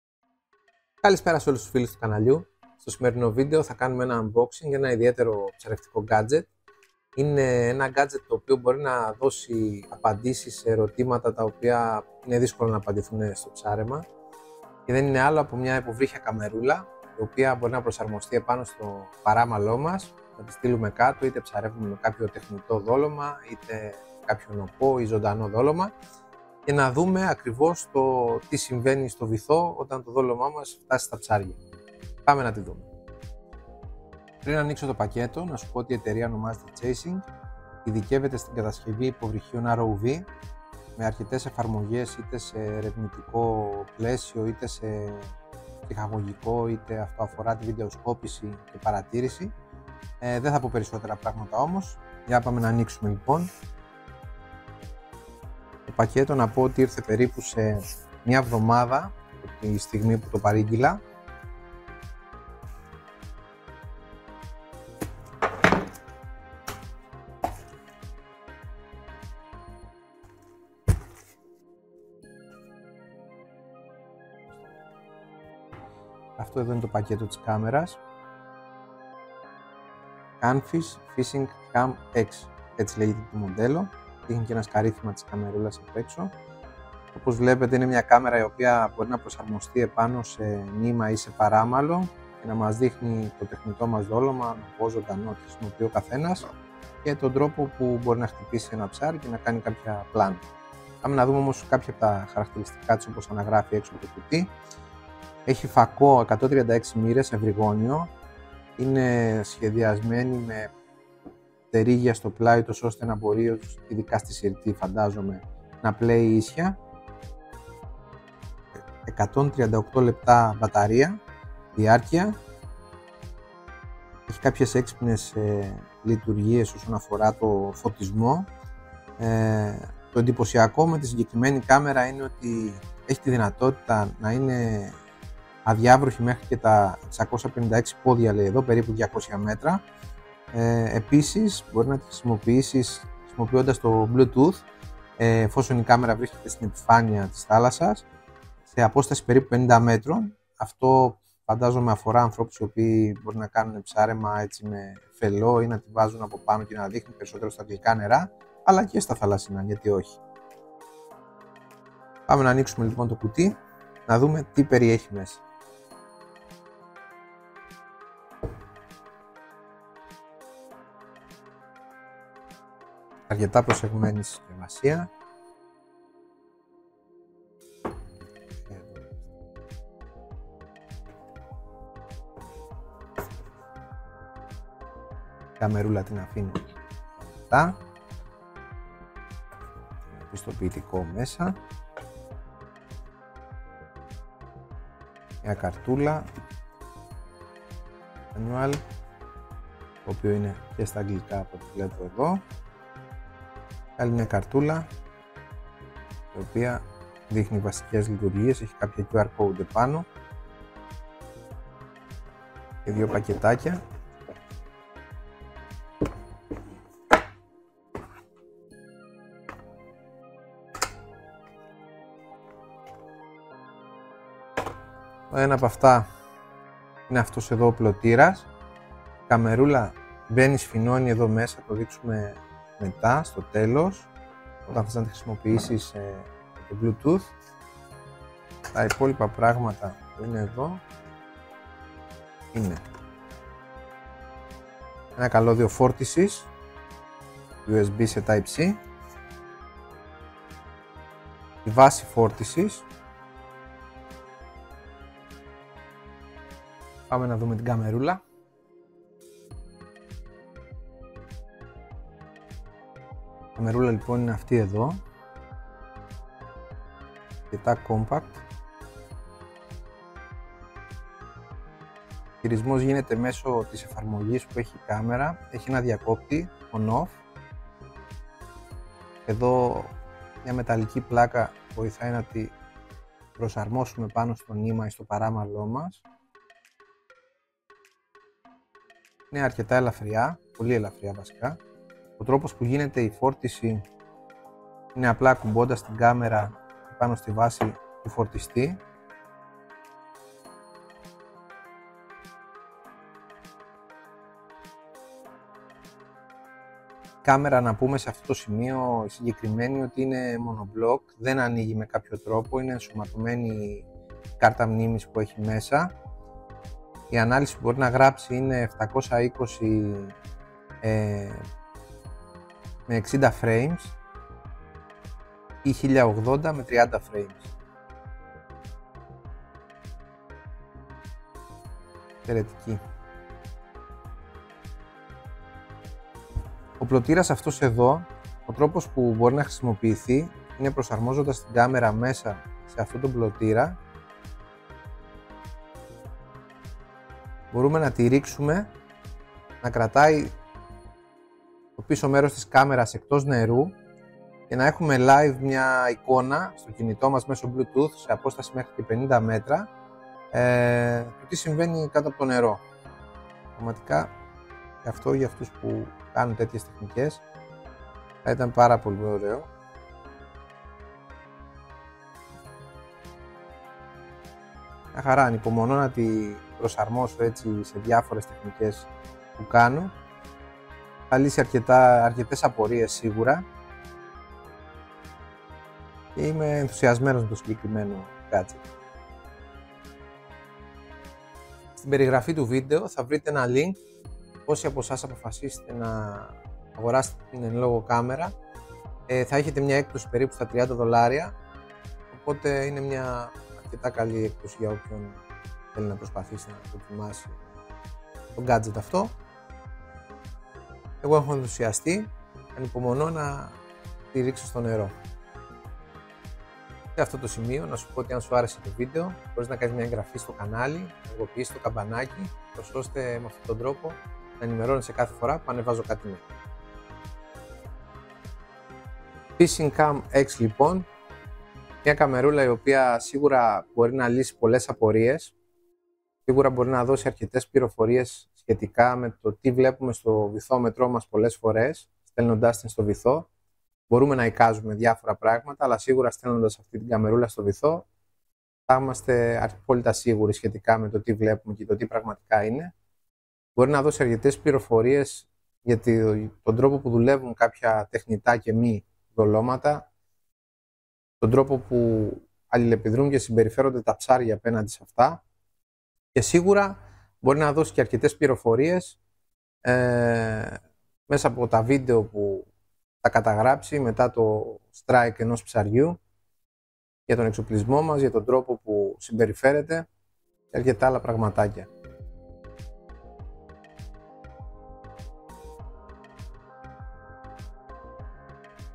Κάνω unboxing την καλύτερη κάμερα ψαρέματος.
Σε αυτό το βίντεο θα δουμε την κάμερα Canfish FishingX που μπορεί να προσαρμοστεί στο νήμα η την πετονιά και να καταγράψει πλανα από το τεχνητο, νωπο η και ζωντανο δόλωμα που χρησιμοποιούμε.
Η χρήση της είναι πολύ απλη, μεσο της εφαρμογής που παρέχει η εταιρεία chasing.
Ένα από τα πιο εντυπωσιακά χαρακτηριστικά της είναι ότι  μπορεί να παραμείνει στεγανη  σε βαθος 200μ.

Αν θέλεις  να την παραγγείλεις πιο κάτω θα βρεις το σύνδεσμο.

 Βαλε τον κωδικό q8iJAt8rs  και θα εχεις έκπτωση 30$.

Η διαδικασία είναι απλη χωρίς τελωνείο και αρκετά γρήγορο shipping! (5-10) ημέρες.